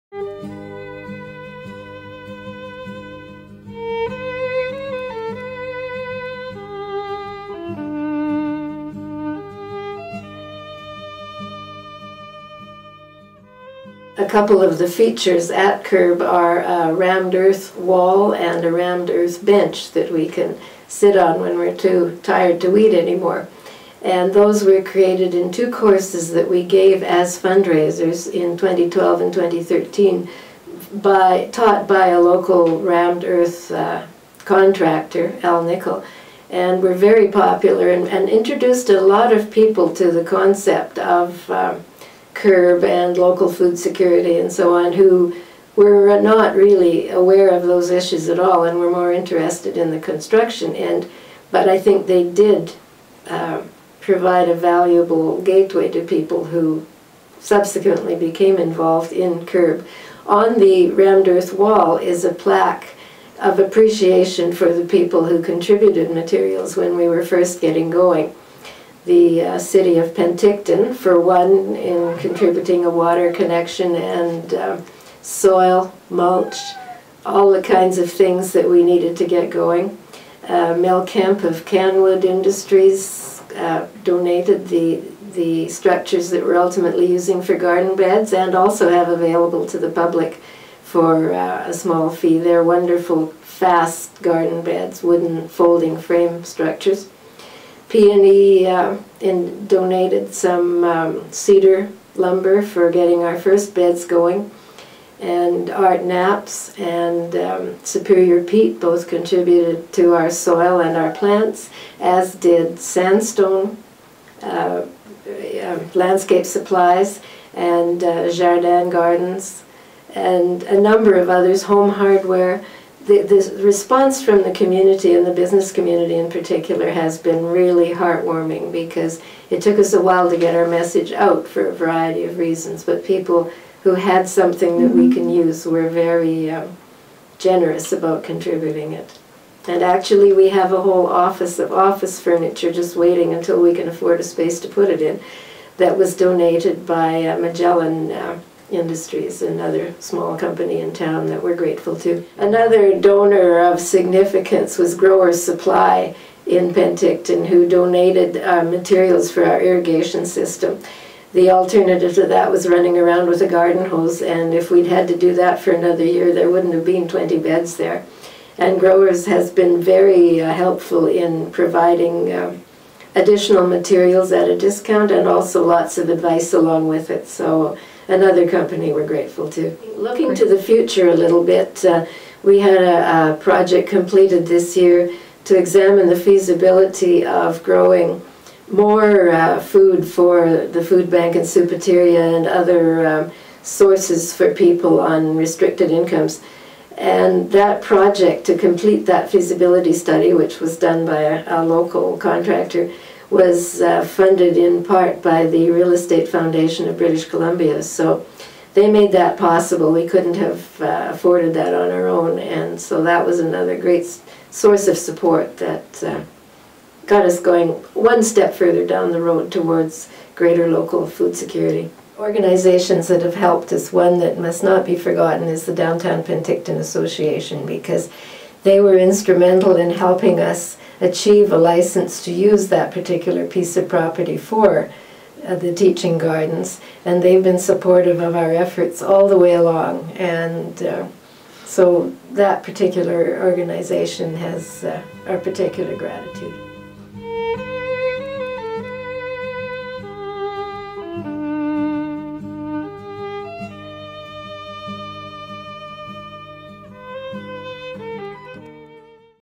PUAA thanks
There has been an outpouring of help from the community to help make our vision a success. In this video we thank the businesses and organizations that donated skills and materials to create the teaching gardens. Right from the start there were a great number of private individuals who rolled up their sleeves to pull weeds, build raised beds, put the garden to sleep for the winter and all points in between. Thank you to everyone for your community spirit...we couldn't have done it without you. We want to keep you up to date with all of the great things we have in store for the future. Get involved...stay involved. Drop us a line and happy gardening. Video produced by Rita Montgomery